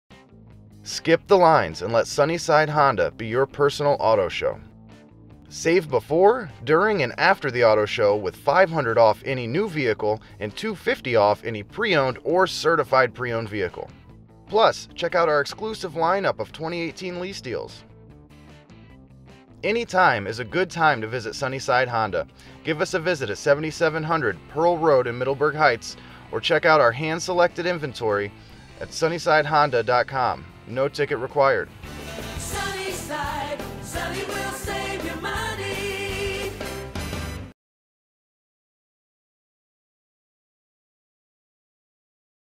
Sunnyside Honda - Auto Show 2018
Skip the lines and come to Sunnyside Honda; your personal auto show. Save before, during and after the auto show with $500 off any new vehicle and $250 off any pre-owned or certified pre-owned vehicle. Also, check out our exclusive line up of 2018 lease deals.